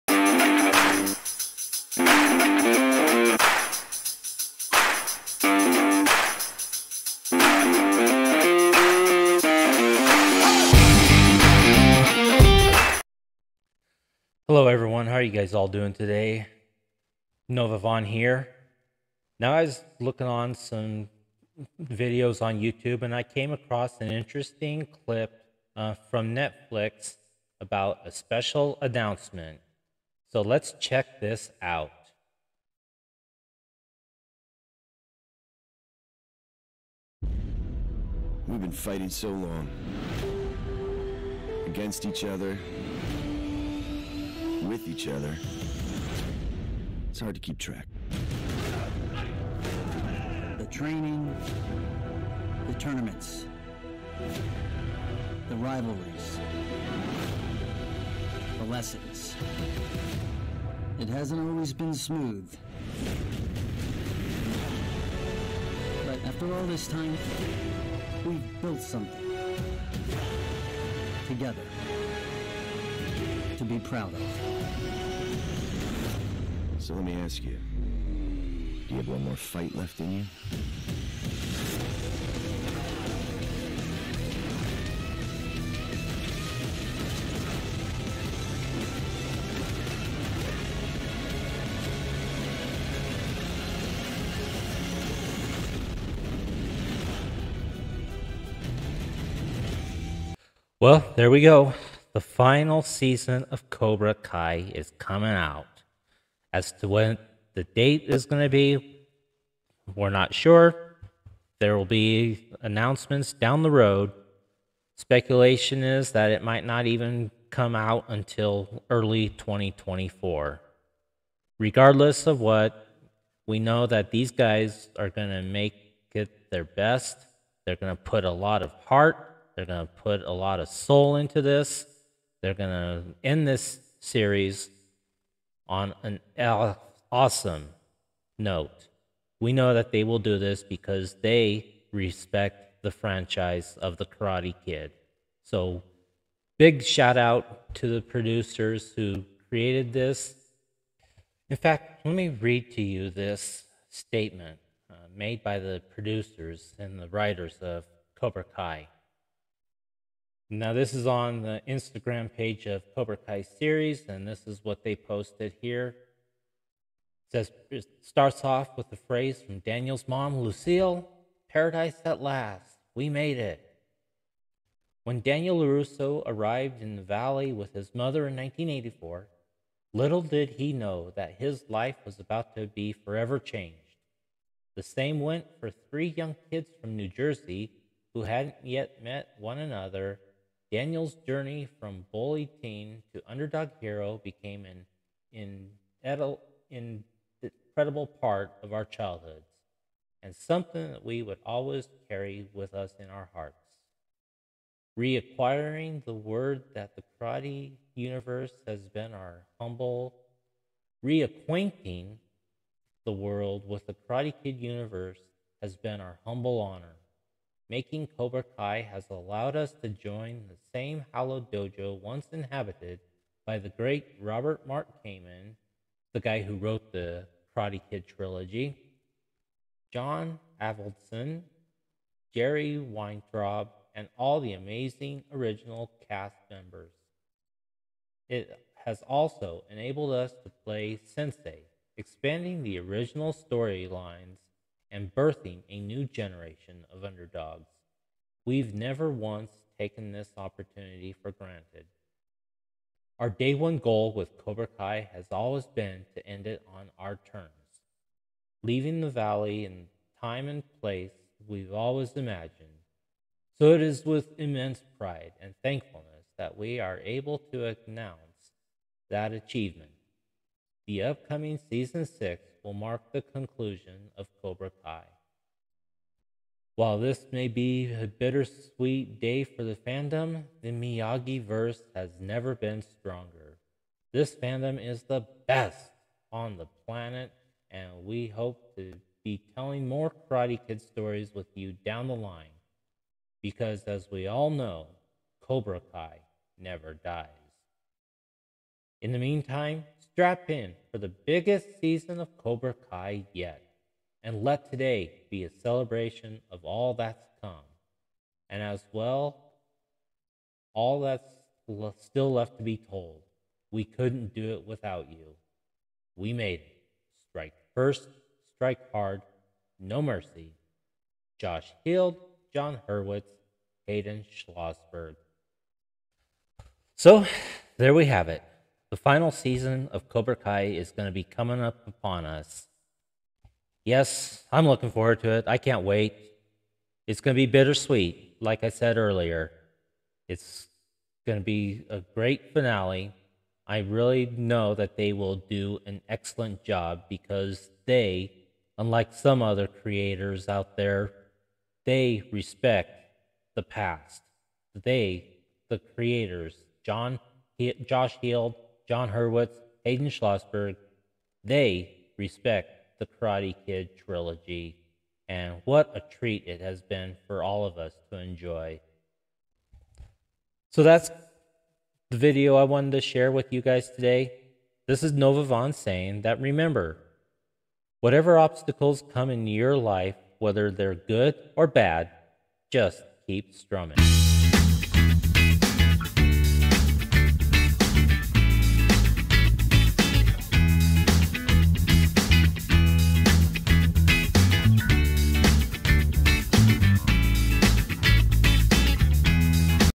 Announcement On Cobra Kai
Today Netflix made an announcement regarding season 6 of Cobra Kai.